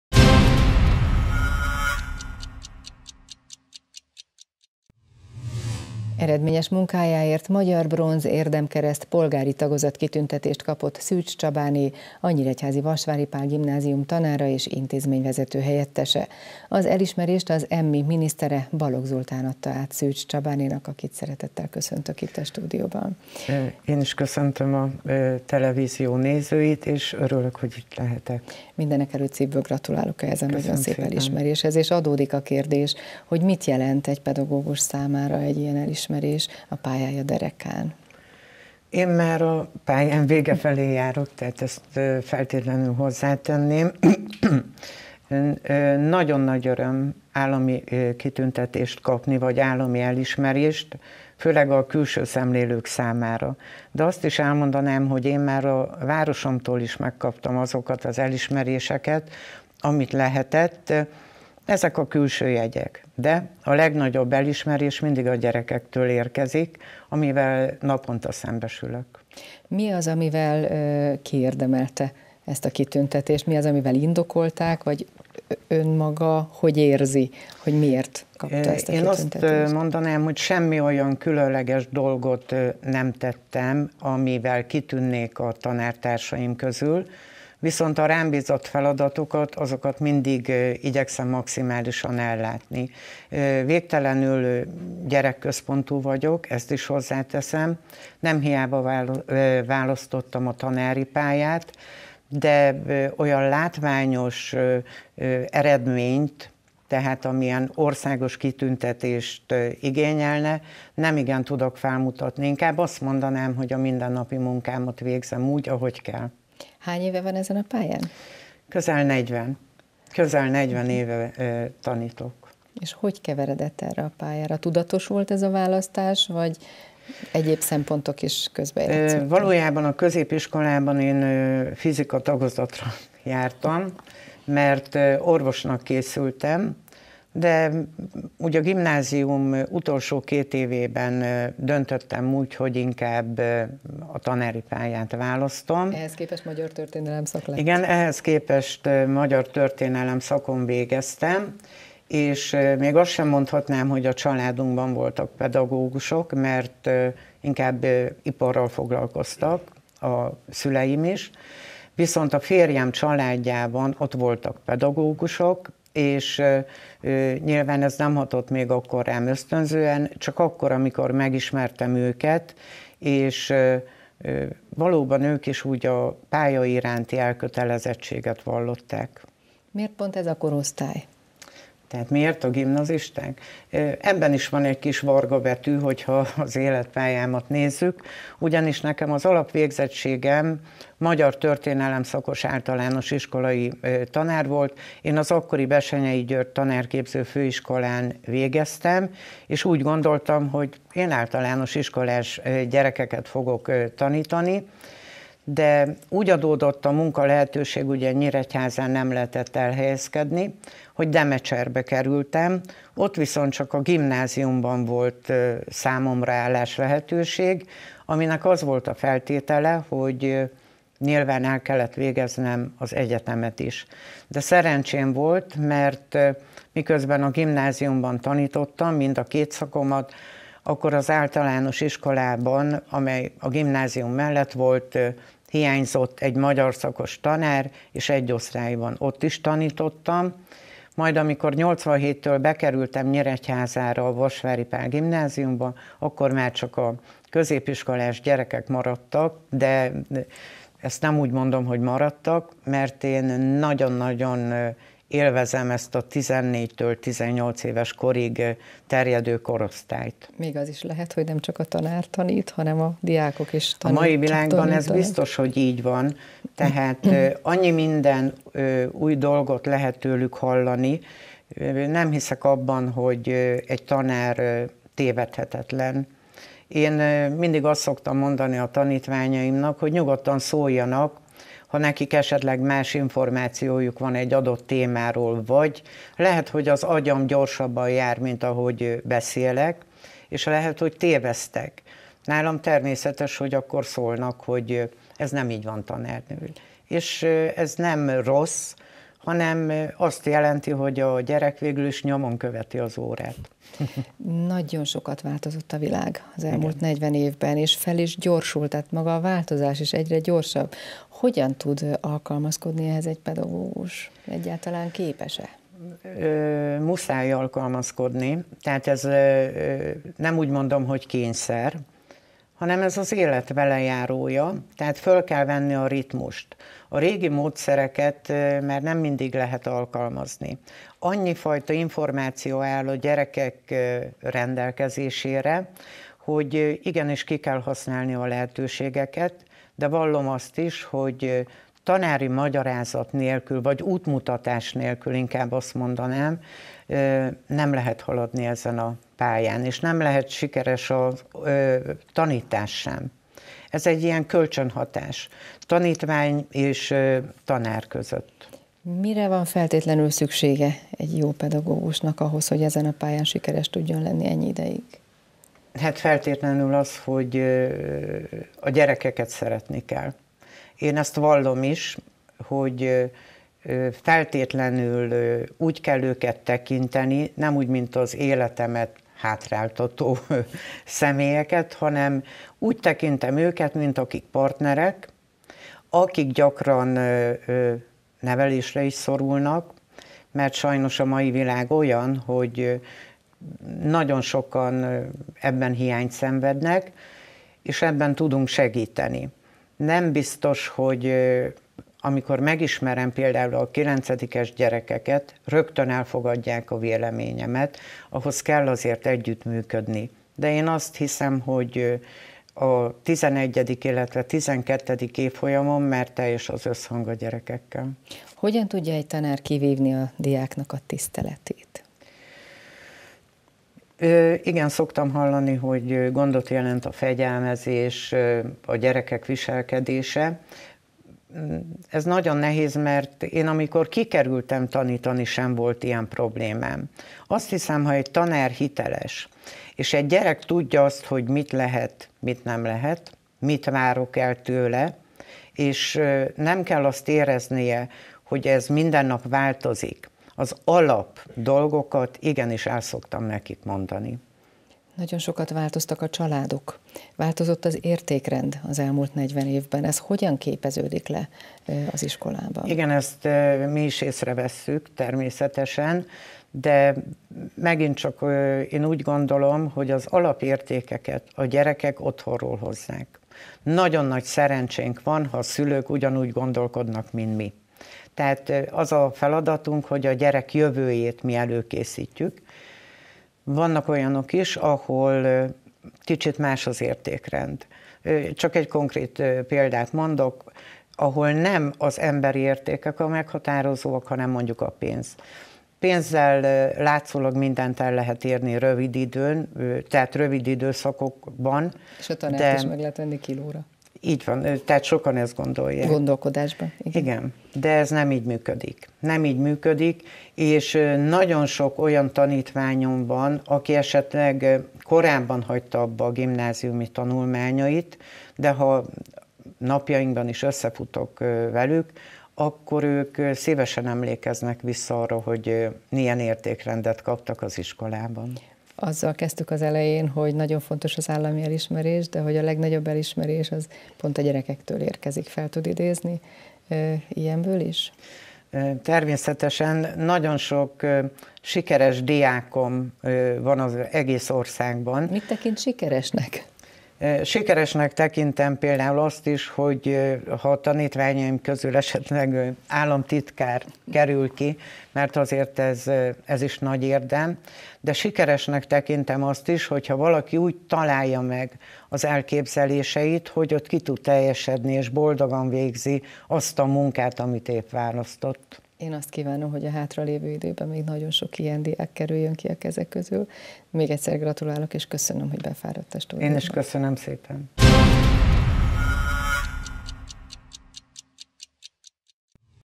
35 perc / Elismerés - Kölcsey Tv
Állami kitüntetést kapott a Vasvári intézményvezető-helyettese.Eredményes munkájáért Magyar Bronz Érdemkereszt polgári tagozat kitüntetést kapott Szűcs Csabáné, a Nyíregyházi Vasvári Pál Gimnázium tanára és intézményvezető-helyettese. Az elismerést az EMMI minisztere, Balog Zoltán adta át Szűcs Csabánénak.